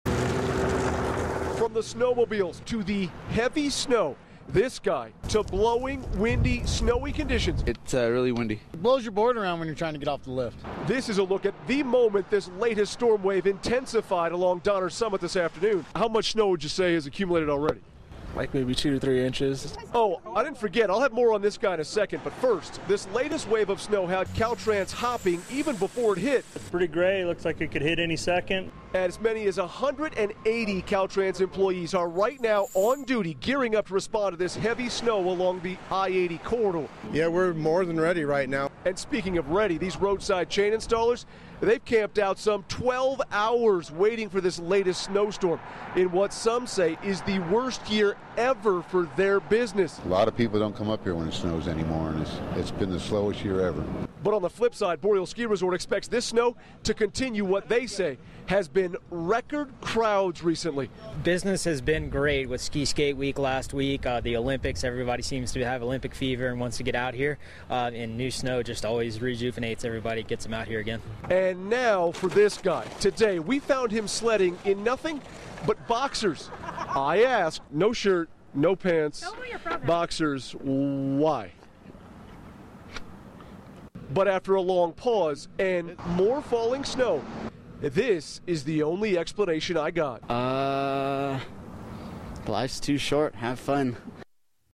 Shirtless In The Snow? 'Life's Too Short'
A shirtless, pantless sledder hit the Sierra Tuesday, and his only explanation was, "Life's too short, have fun."